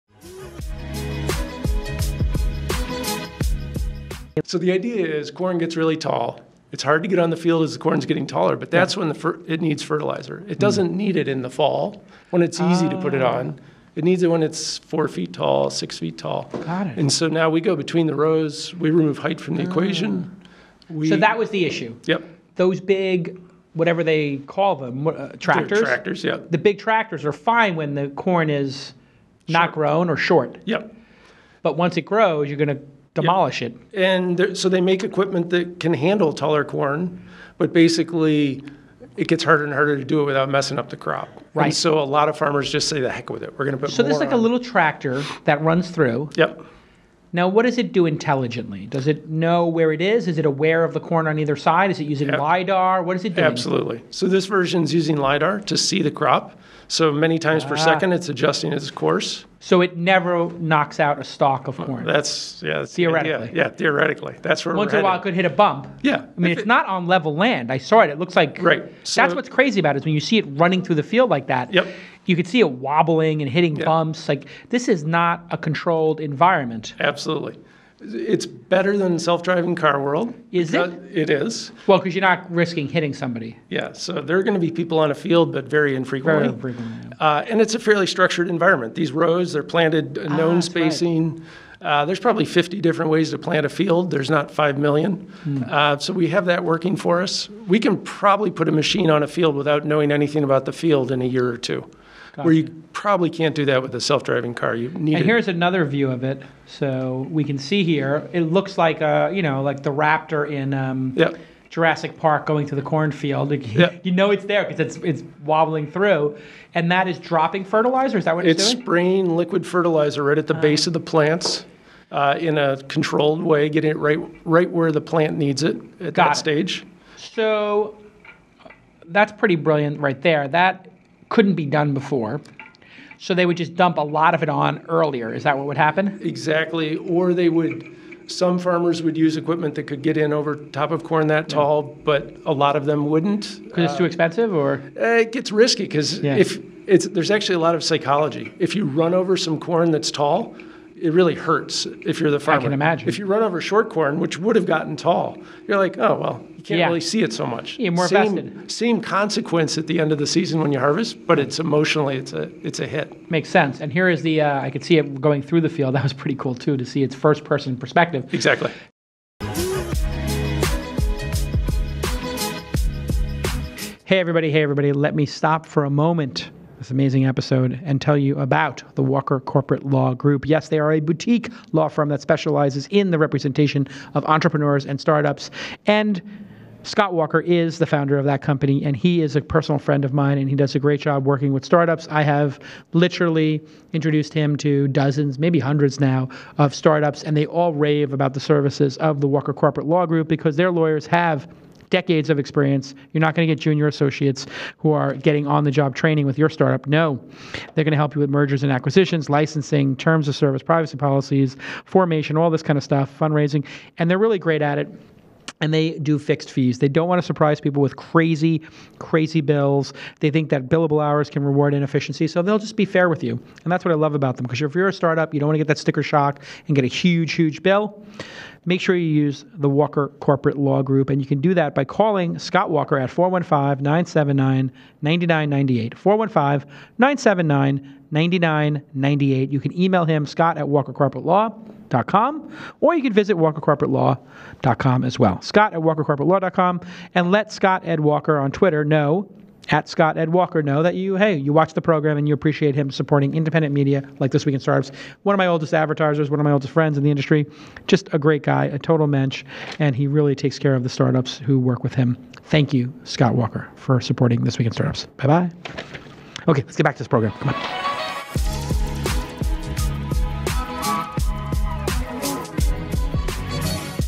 Founder Kent Cavender-Bares shows how Rowbots robotic farming fertilizes between rows
.@kbares Founder @rowbotsystems shows @jason how his farming robots fertilize crops between rows-THX @ScottEdWalker

Science rules! Rowbots transforms agriculture with robotic farming & Experiment.com crowdfunds cutting-edge research

Special thanks to the members of the TWiST Backchannel!